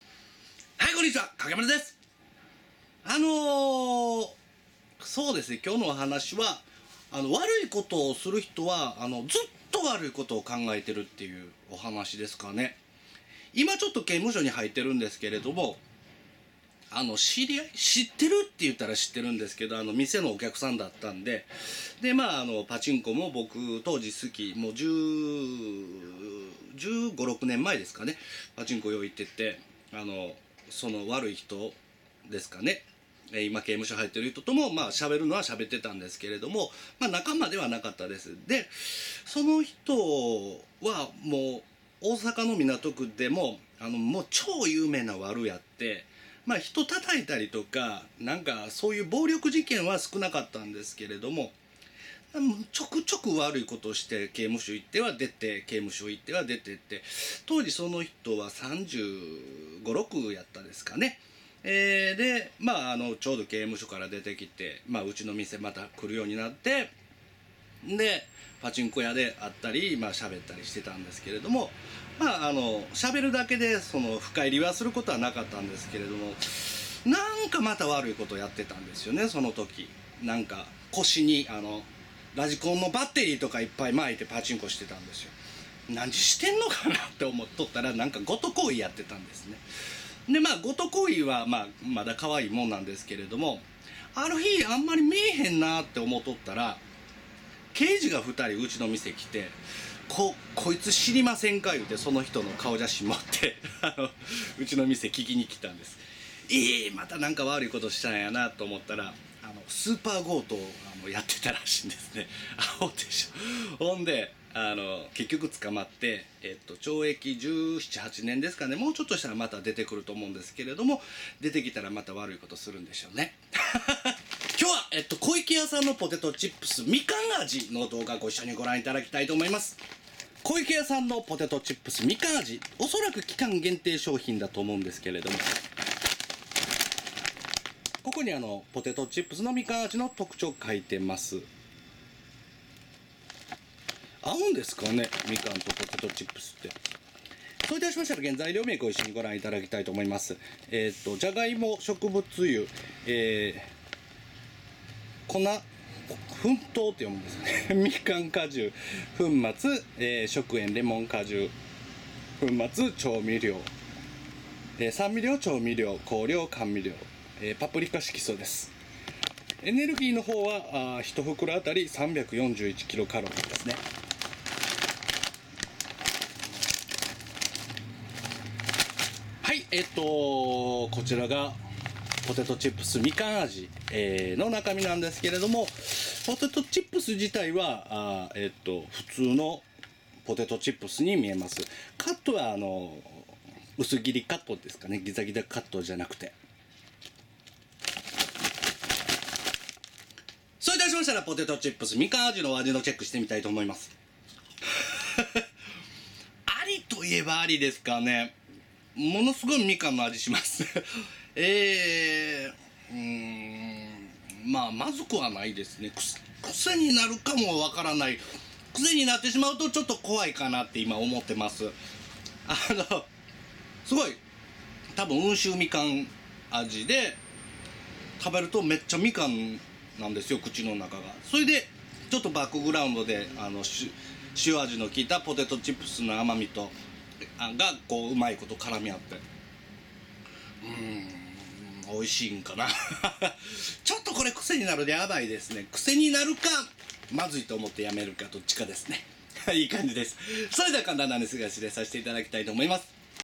コイケヤ ポテトチップス みかん味 レビュー
コイケヤ ポテトチップス みかん味を購入し食べた感想を解説した動画です。